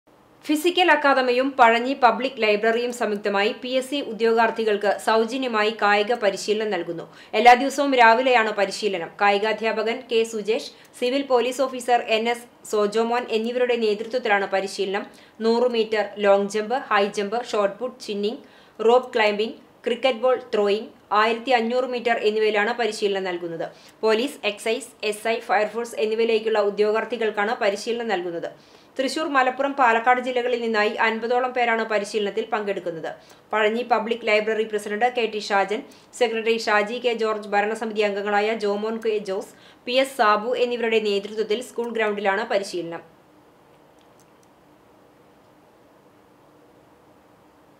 പിഎസ് സി ഉദ്യോഗാര്‍ഥികള്‍ക്ക് സൗജന്യ കായിക പരിശീലനം
ഫിസിക്കല്‍ അക്കാദമിയും പഴഞ്ഞി പബ്ലിക്  ലൈബ്രറിയും സംയുക്തമായി പിഎസ് സി ഉദ്യോഗാര്‍ഥികള്‍ക്ക് സൗജന്യമായി കായിക പരിശീലനം നല്‍കുന്നു. എല്ലാദിവസവും രാവിലെയാണ് പരിശീലനം. കായികാധ്യാപകന്‍  കെ.സുജേഷ്, സിവില്‍ പൊലീസ് ഓഫിസര്‍ എന്‍.എസ്. സോജോമോന്‍ എന്നിവരുടെ നേതൃത്വത്തിലാണ് പരിശീലനം.